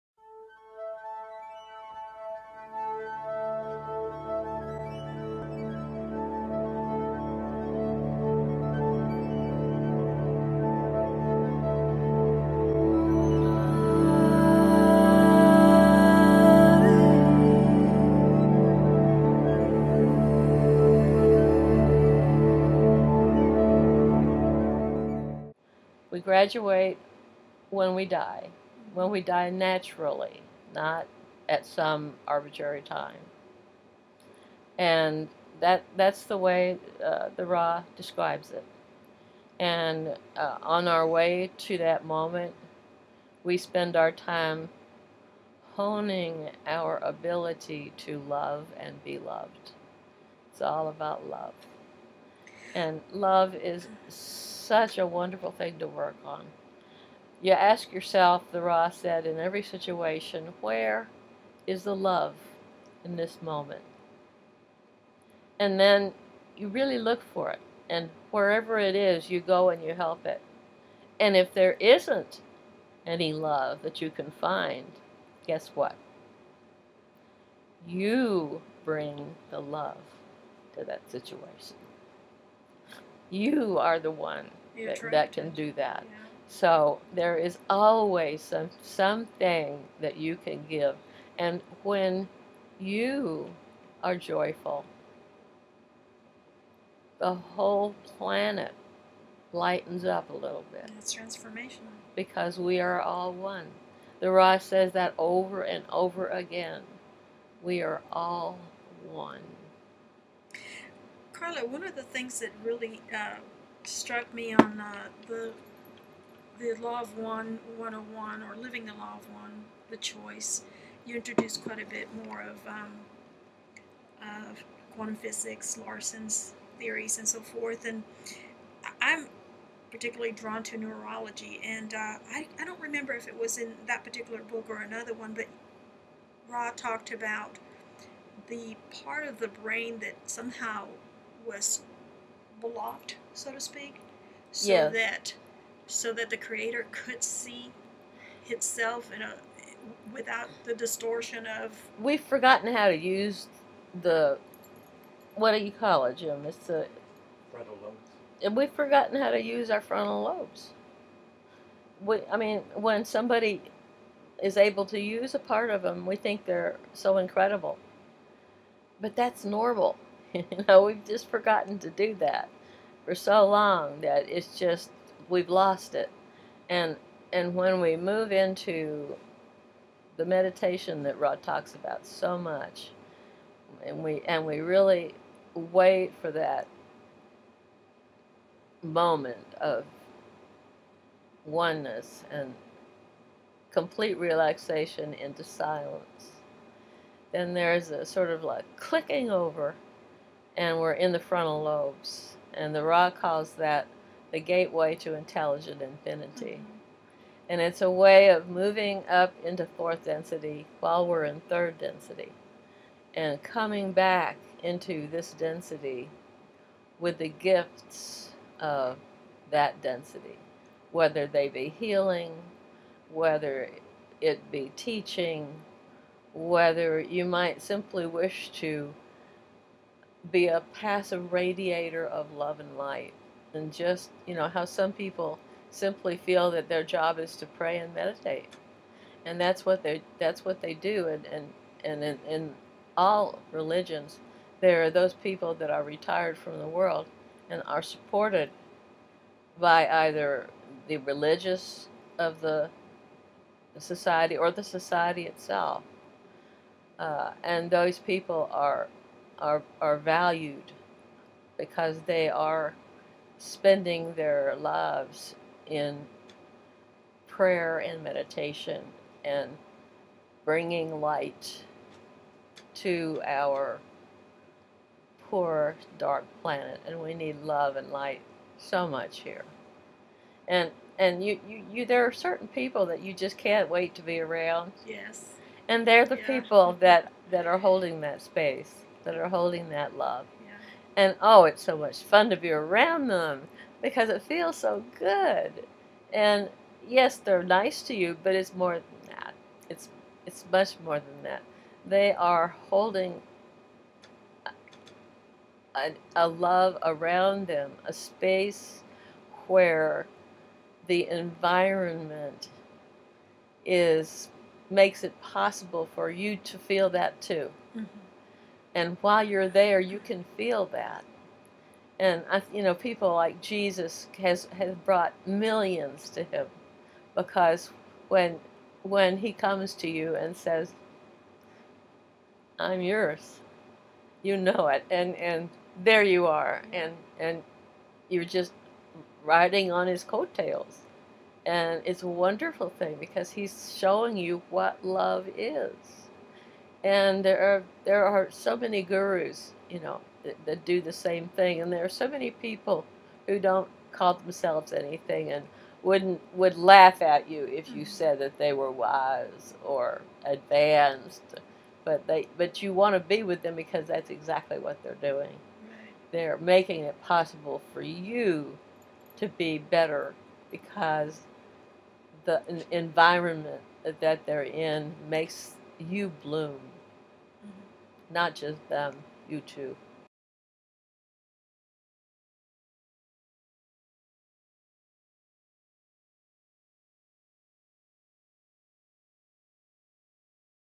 Interview with Carla Rueckert - Denise Wilbanks (Pt 7) - Love and Meditation
HuffPo blogger Denise Wilbanks interviews Carla L. Rueckert about spirituality and the Law of One.

Part 7 - The Steps of Light